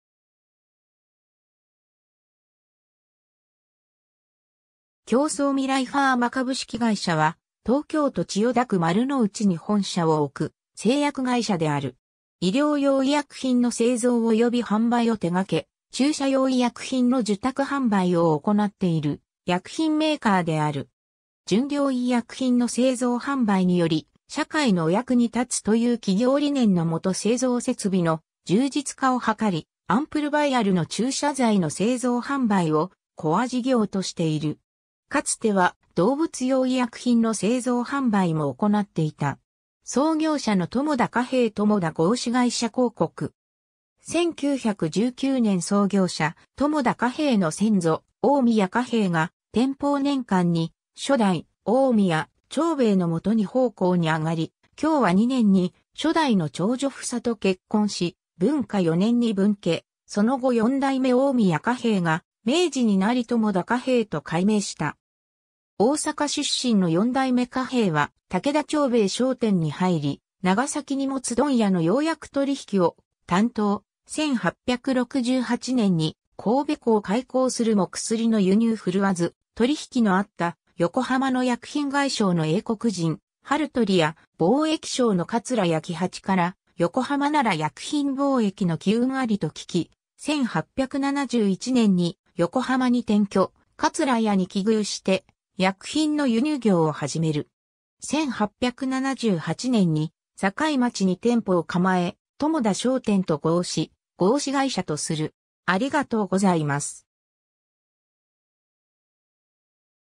共創未来ファーマ
共創未来ファーマ株式会社（きょうそうみらいファーマ）は、東京都千代田区丸の内に本社を置く製薬会社である。
 医療用医薬品の製造及び販売を手がけ、注射用医薬品の受託販売を行っている薬品メーカーである。
「純良医薬品の製造販売により社会のお役に立つ」という企業理念のもと製造設備の充実化を図り、アンプル･バイアルの注射剤の製造販売をコア事業としている。
かつては動物用医薬品の製造販売もおこなっていた。
 創業者の友田嘉兵衛 友田合資会社広告。
1919年 創業者友田嘉兵衛の先祖、近江屋嘉兵衛が天保年間に初代近江屋長兵衛のもとに奉公に上がり、享和2年（1802年）に初代の長女ふさと結婚し、文化4年（1807年）に分家、その後4代目近江屋嘉兵衛が明治になり友田嘉兵衛と改名した。
大阪出身の4代目嘉兵衛は武田長兵衛商店に入り、長崎荷物問屋の洋薬取引を担当、1868年に神戸港開港するも薬の輸入振るわず、取引のあった横浜の薬品外商の英国人ハルトリや貿易商の桂屋喜八から横浜なら薬品貿易の機運ありと聞き、1871年に横浜に転居、桂屋に寄寓して薬品の輸入業を始める。
1878年に境町（現・横浜市中区日本大通）に店舗を構え、友田商店と号し、合資会社とする。